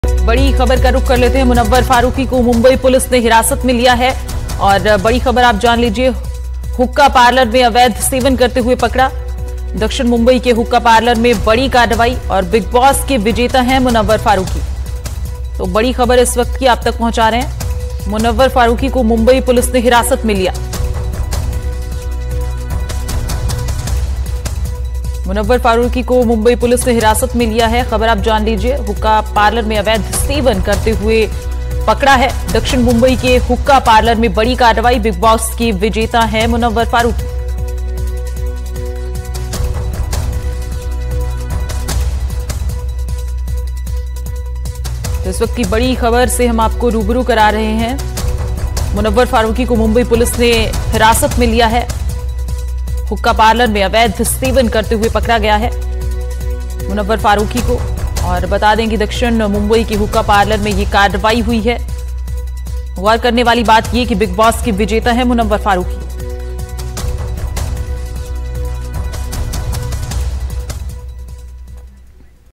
Munawar Faruqui Detained BREAKING NEWS: मुनव्वर को Mumbai Police ने हिरासत में लिया | NDTV India
Munawar Faruqui Detained: मुंबई पुलिस (Mumbai Police) ने मंगलवार रात एक हुक्का पार्लर में छापेमारी कर स्टैंड-अप कॉमेडियन और बिगबॉस फेम मुनव्वर फारूकी को हिरासत (Munawar Faruqui Detained) में ले लिया था. हुक्का पार्लर में तंबाकू के सेवन की जानकारी मिलने के बाद पुलिस वहां छापेमारी (Hookah Parlour Raid) करने पहुंची थी. हिरासत में लिए गए लोगों में मुनव्वर फारूकी का नाम भी शामिल है. रेड से जुड़े एक अधिकारी ने बताया, “हमारी टीम ने हुक्का के नाम पर तंबाकू के इस्तेमाल होने की जानकारी मिलने के बाद मुंबई के हुक्का बार पर छापा मारा, वहां से मिली हुईं चीजों की जांच के बाद हिरासत में लेकर मामला दर्ज किया गया है. पकड़े गए लोगों में मुनव्वर फारूकी भी शामिल था."